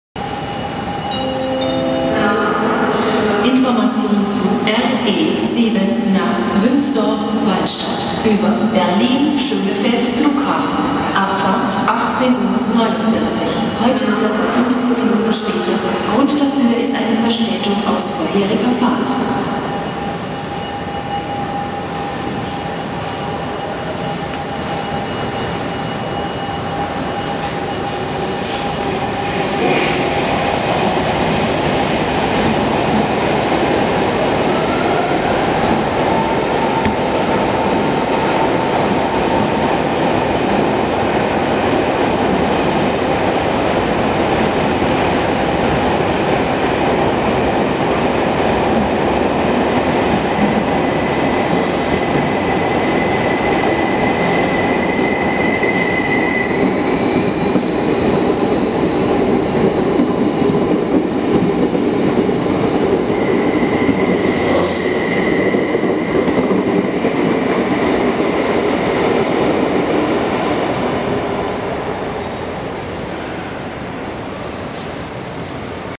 Ausfahrt ODEG KISS (ET 445) in Bln. Friedrichstraße
Yay, jetzt fährt der ODEG auch auf der Stadtbahn ... Ja, Privatbahnen haben jetzt auch einen Weg in die City gefunden :=)